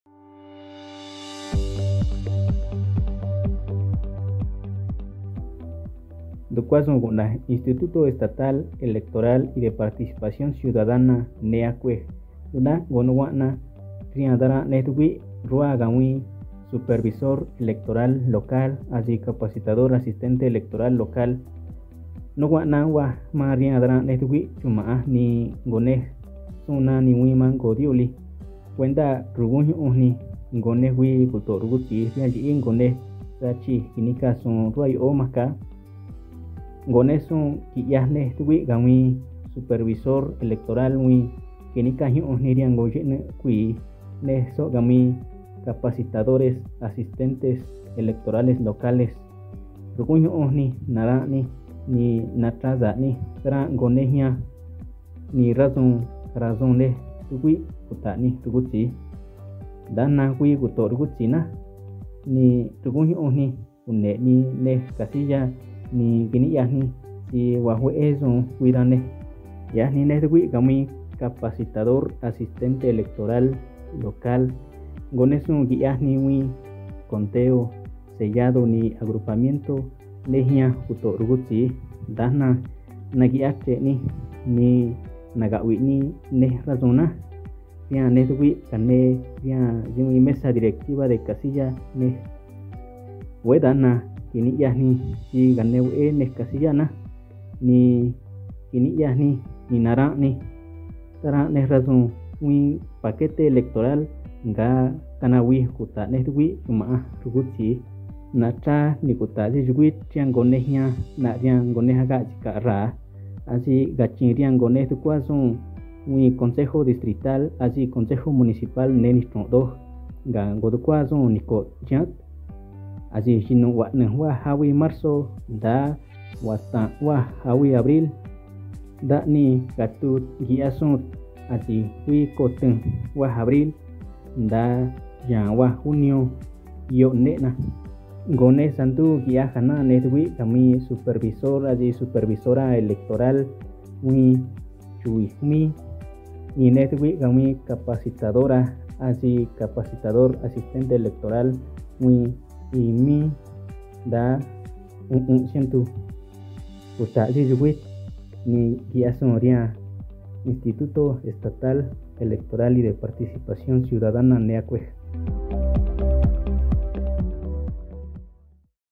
Spot en Triqui - Convocatoria SEL y CAEL para las Elecciones Oaxaca 2024
Te invitamos a participar en las EleccionesOaxaca2024 como persona Supervisora Electoral o Capacitadora Asistente Electoral.